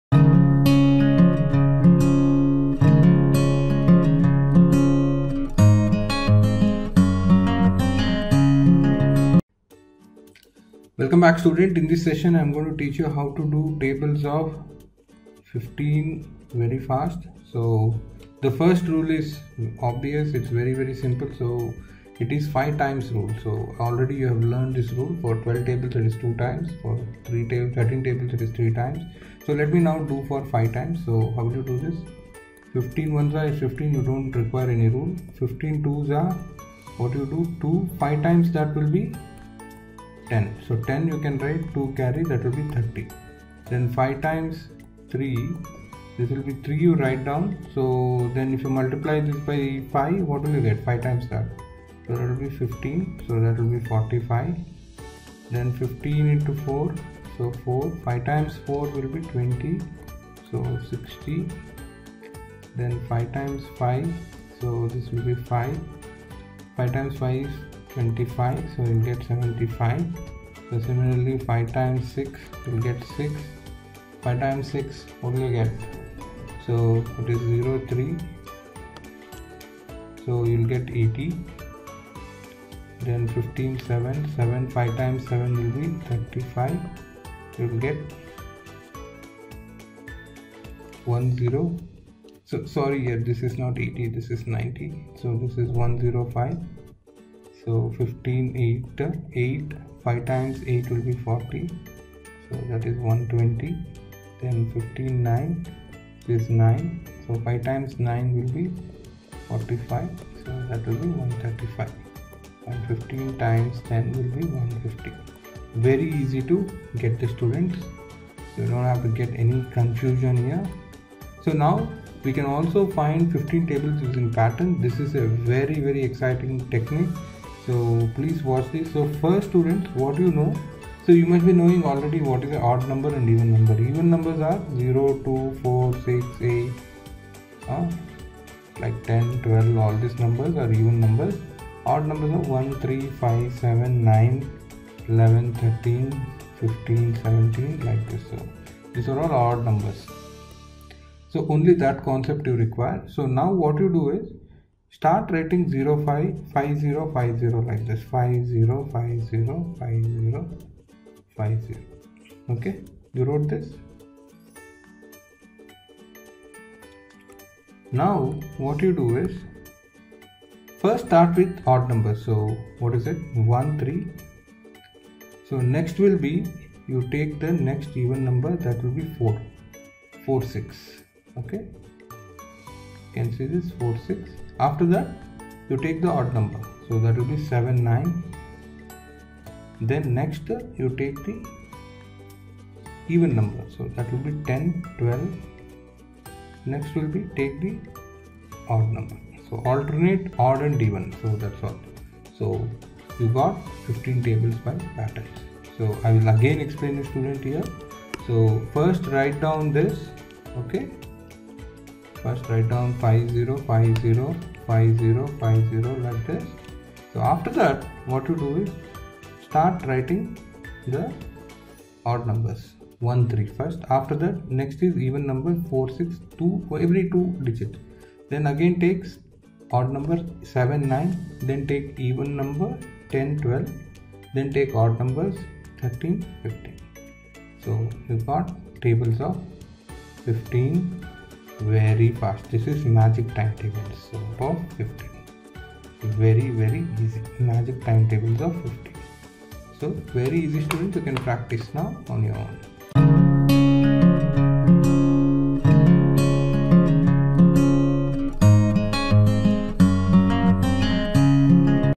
Learn 15 multiplication table trick | Easy and fast way to learn | Math Tips and Tricks
In this video, students will learn how to get multiplication times tables tricks in fun and simple way. Best teaching by IITians and Experts.